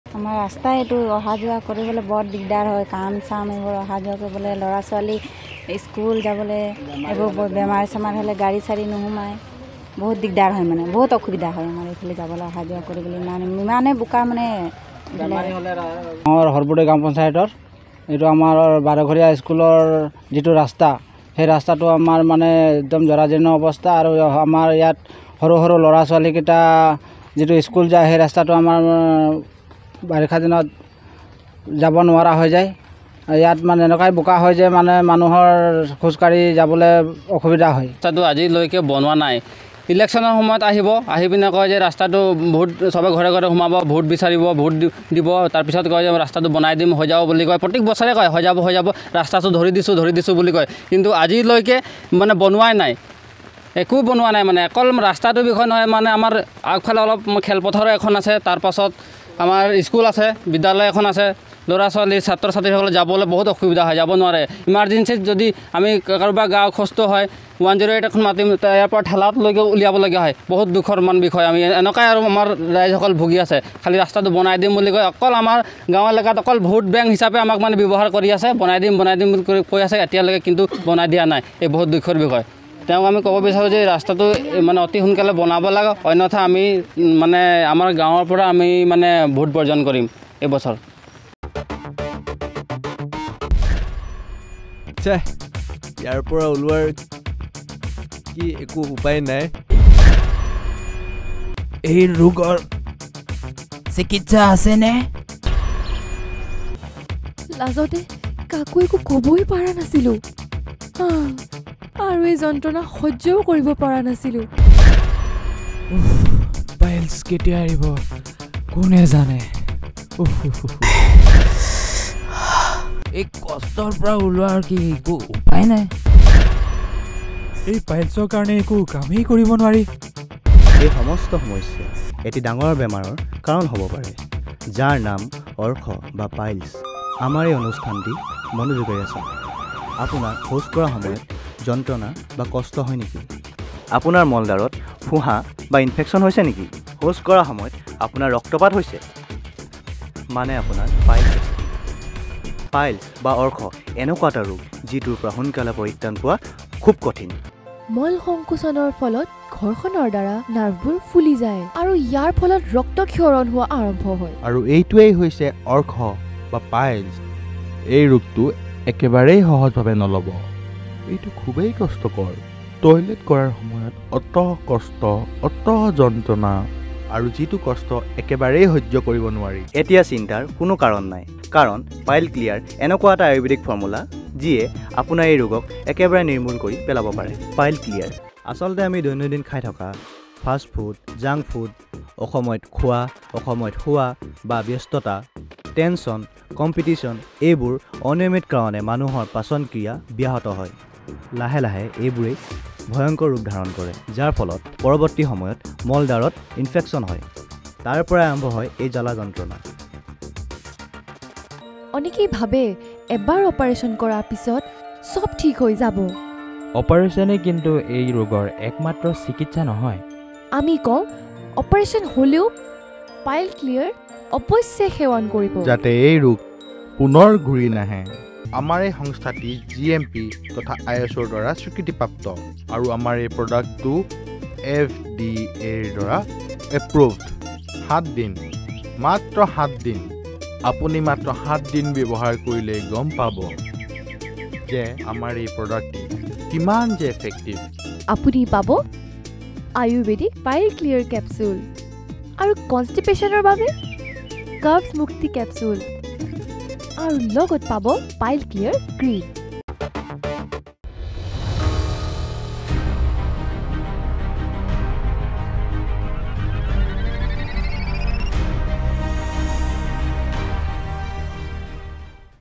বিধায়ক যোগেন মোহনৰ নিজ সমষ্টিৰ পথৰ দুৰৱস্থা
বিধায়ক যোগেন মোহনৰ নিজ সমষ্টিৰ পথৰ দুৰৱস্থা, মাহমৰা সমষ্টিৰ সৰ্বোদয় গাঁও পঞ্চায়তৰ এটা বিধ্বস্ত পথে জ্বলা-কলা খোৱাইছে সৰ্বসাধাৰণ ৰাইজক ।